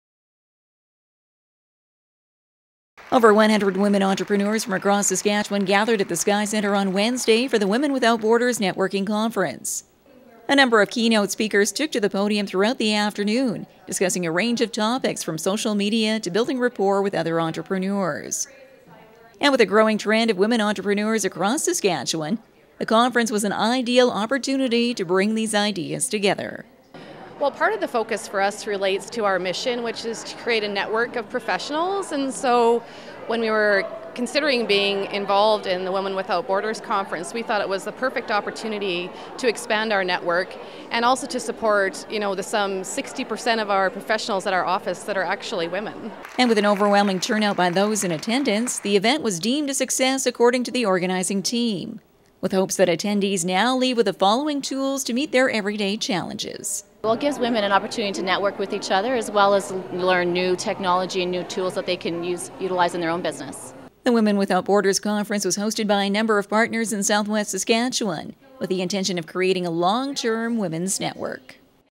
Women Without Borders
Networking for Women Entrepreneurs was the focus of a
one day conference in Swift Current.